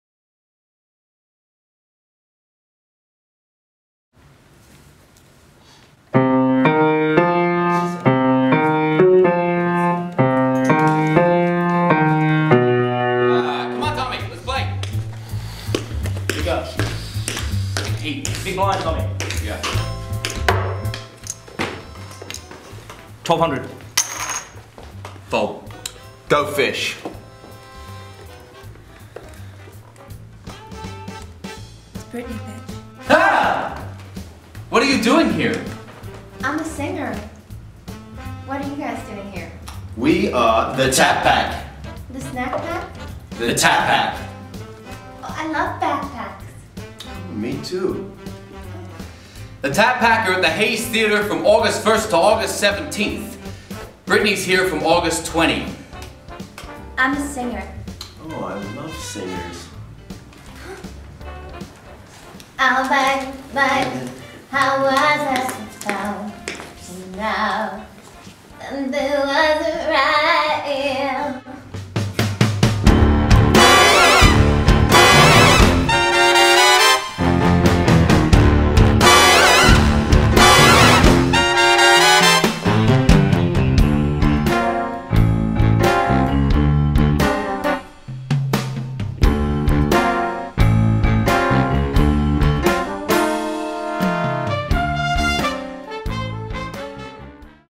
The Tap Pack Video Hayes Britney
The Tap Pack Promoting for Hayes Theatre Co with Christie Whelan!